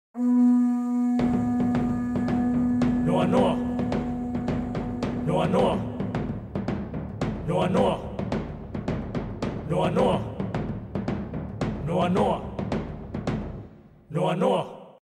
Noa Noa Hawaii Commercial 1
Written, Produced and Scored by Audy Kimura, photographed and edited by Dean Sensui. Hawaiian kapa for opening created and designed by master artist Dalani Tanahy.